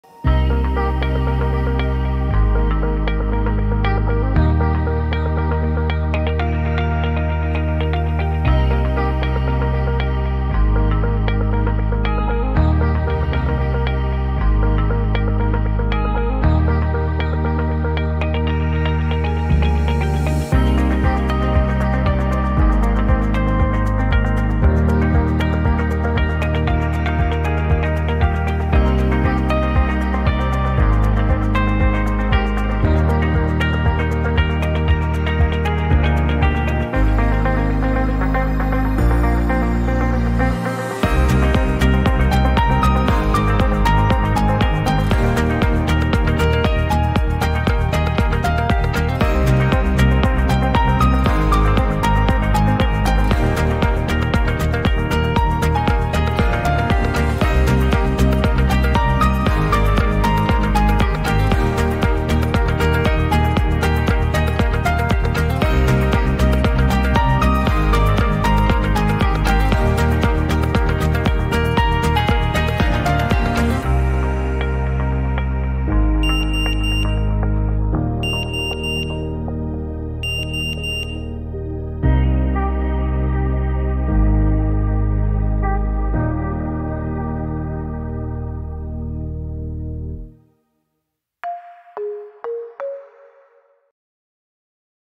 PHILIPS XPERION 6000 UV PILLAR - Долговечное современное освещение
Наш премиальный фонарь Philips Xperion 6000 UV Pillar универсален и предлагает несколько мощных источников света, УФ-свет, высокий CRI и долговечный аккумулятор. Легко находите свой фонарь с помощью функции Find My Device.